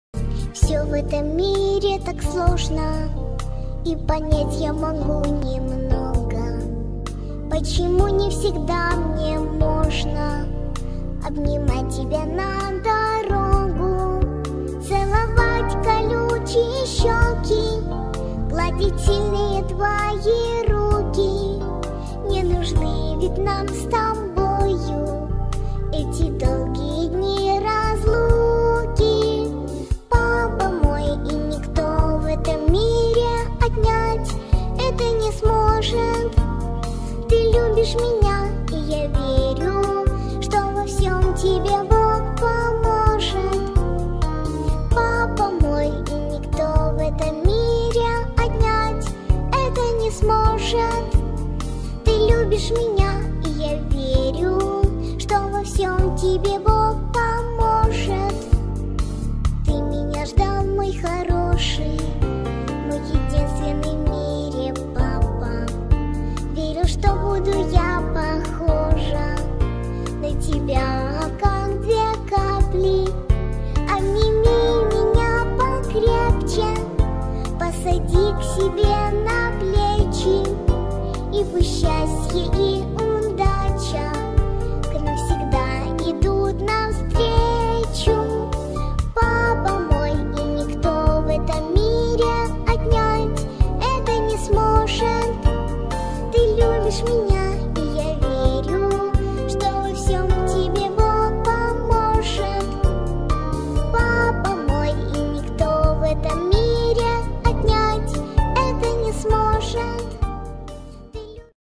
Мой папа самый лучший
Я еще летом обработала несколько видео с праздника "Мой папа самый лучщий!"
Сейчас удаляя лишнюю информацию с диска решила загрузить несколько обработанных видео на канал